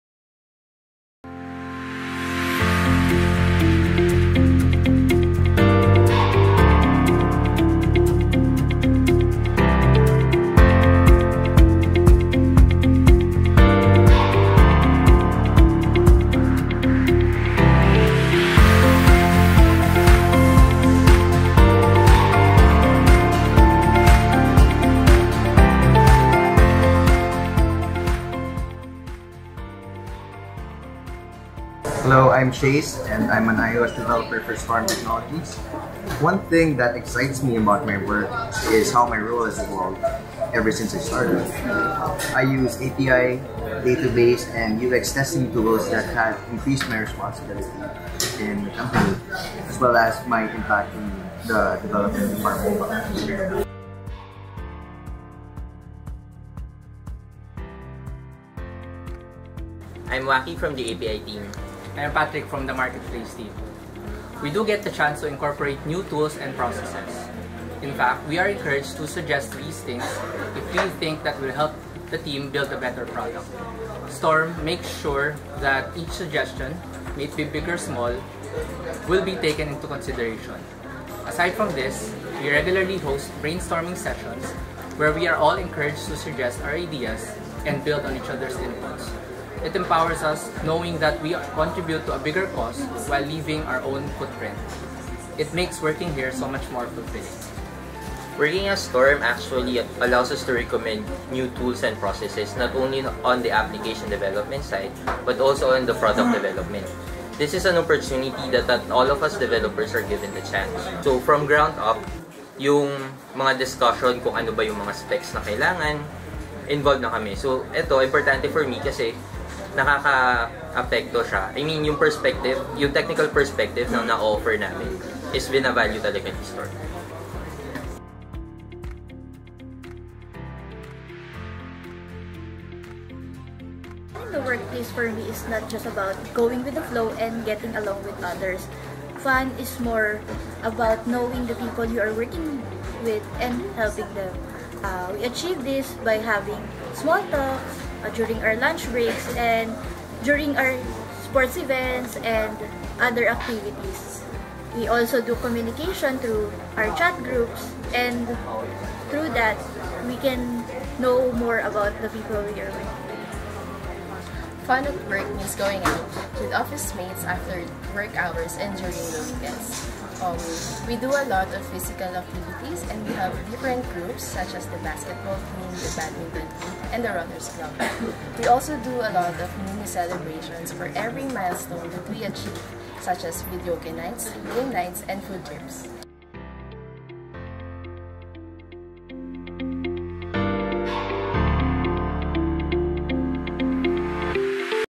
STORM Stories: Research and Development Engineers
One of the things that STORM takes pride in is its technology and the hardworking team of Research and Development Engineers, who keeps the company running at its very core. Never the type to shy away from a challenge, this team holds the constant drive to keep improving the company. Think you got the same driven spirit as they do? Give those fresh eyes and creative minds a try by applying here: storm.tech/Careers